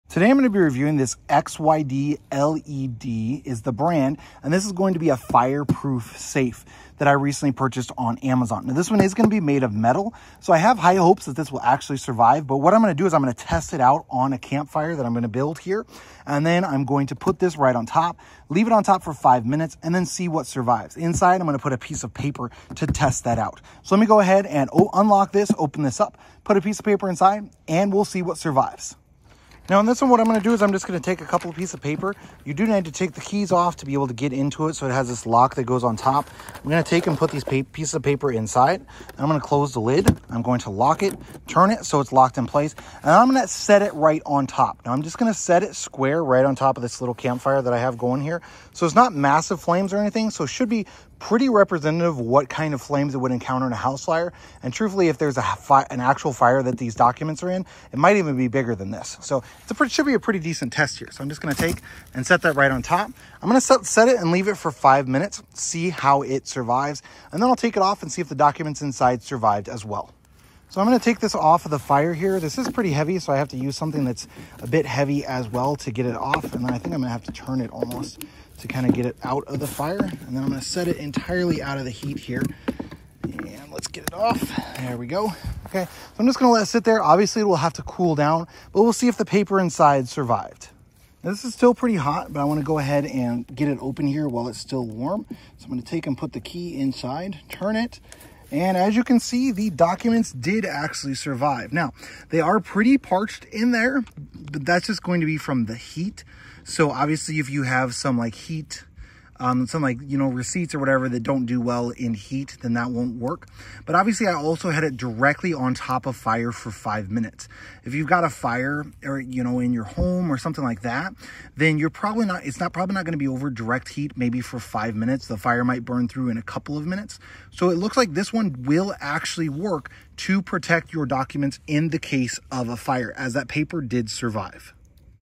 Real Fire Test of XYDled Fireproof Metal Safe/Document Box
In today’s video, I will be reviewing my XYDled Fireproof Metal Document Box!